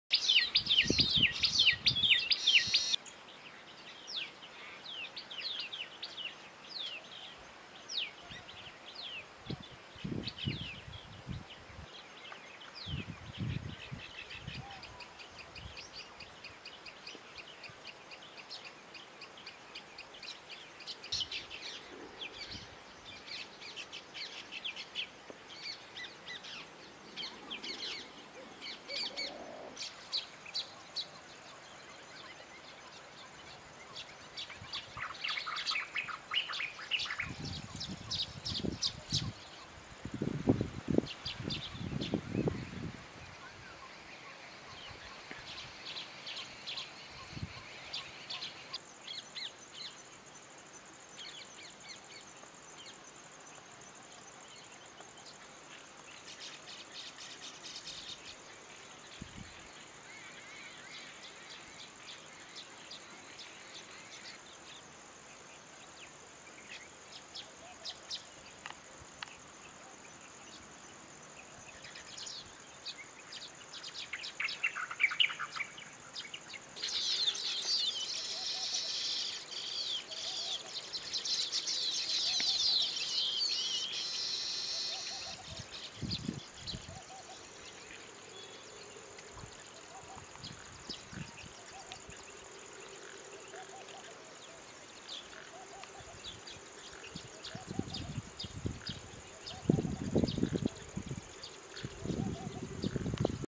Weaver Bird (Baya Weaver) - The King of nest building birds
The baya weaver (Ploceus philippinus) is a weaverbird found across the India and Southeast Asia. Flocks of these birds are found in grasslands, cultivated areas, scrub and secondary growth and they are best known for their hanging retort shaped nests woven from leaves. These nest colonies are usually found on thorny trees or palm fronds and the nests are often built near water or hanging over water where predators cannot reach easily. They are widespread and common within their range but are prone to local, seasonal movements mainly in response to rain and food availability.